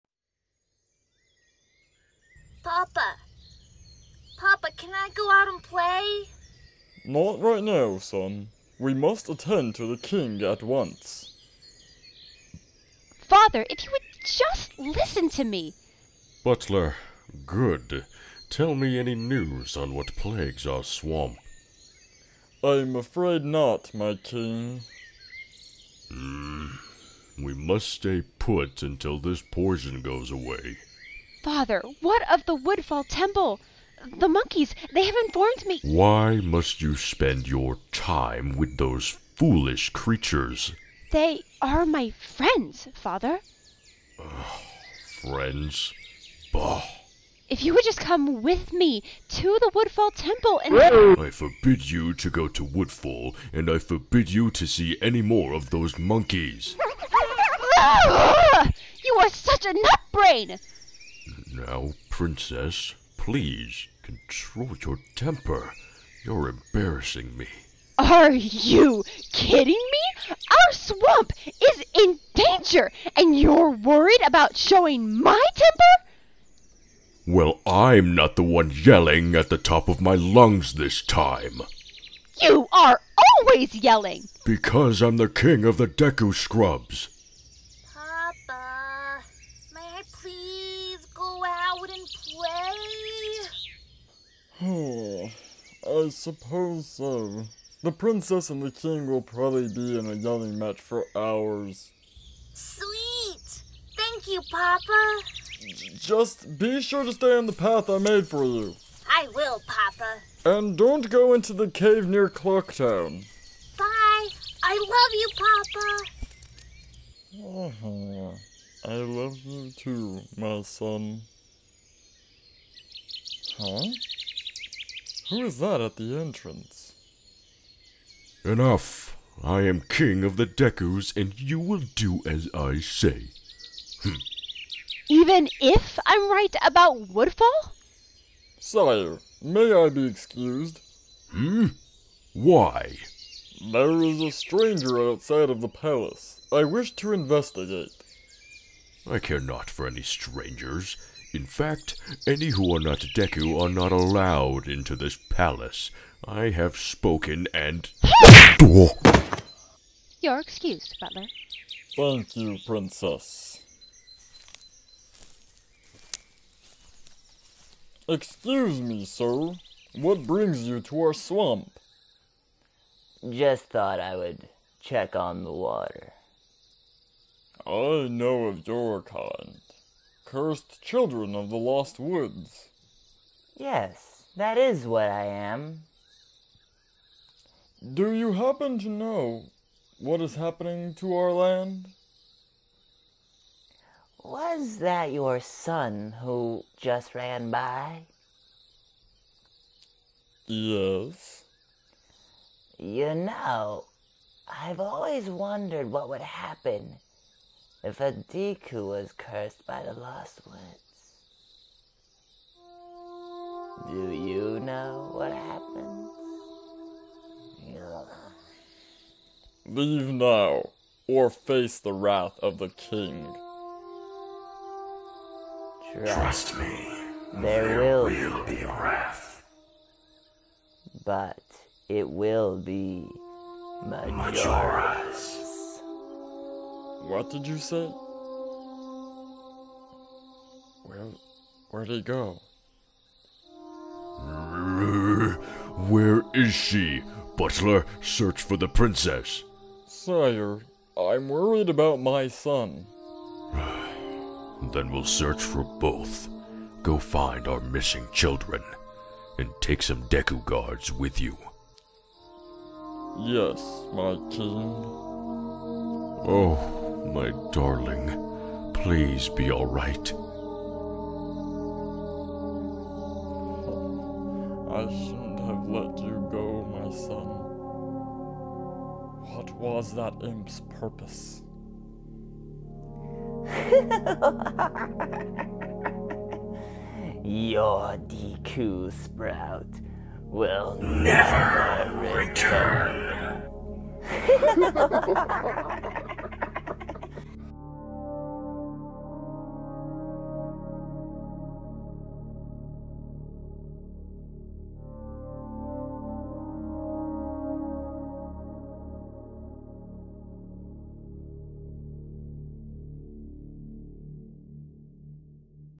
Poisoned Waters, Tales of Termina
The Deku Royal family discuss a strange poisonous horror that plagues their swamp. The Skullkid observes, the Princess and the King in an argument, the Son of the Butler gets impatient and want to play, and the Butler who notices the Imp.